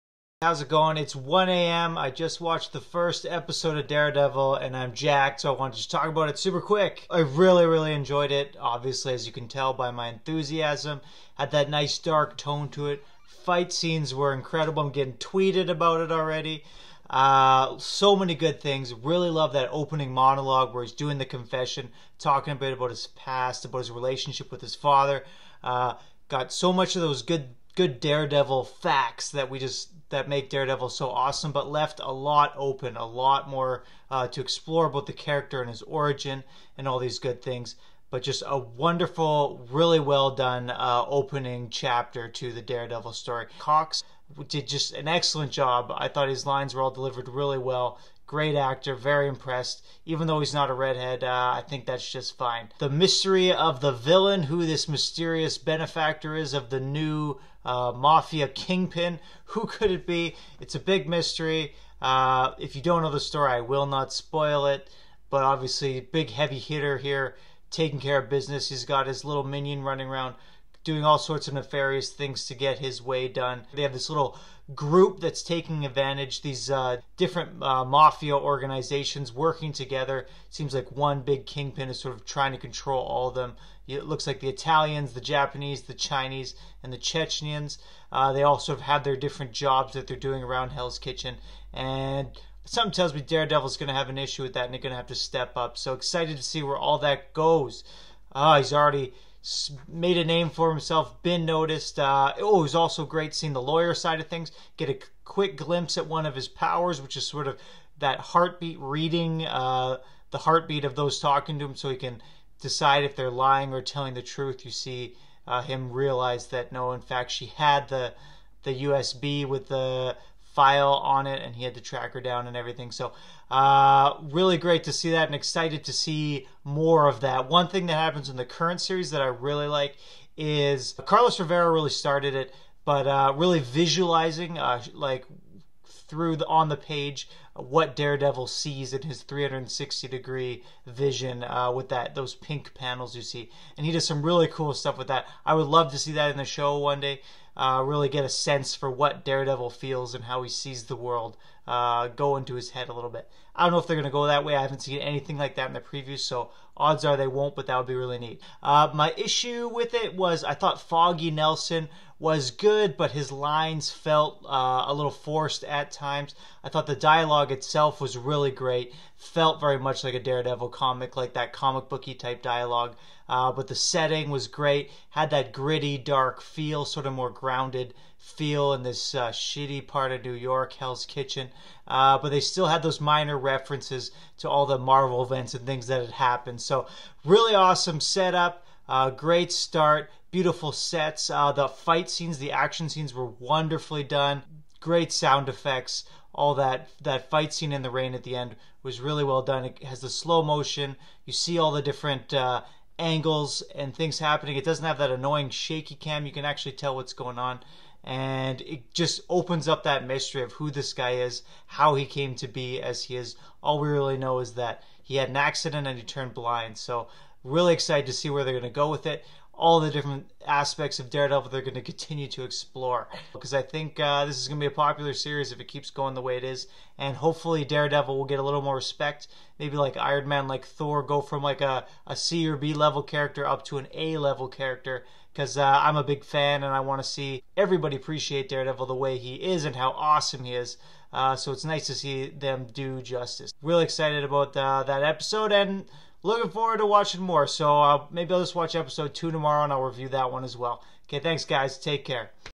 DAREDEVIL Netflix Episode #1 Review! (No Spoilers)
Daredevil comes out swinging easily super passing every other superhero TV show ever made. Yes, even better than My Secret Identity.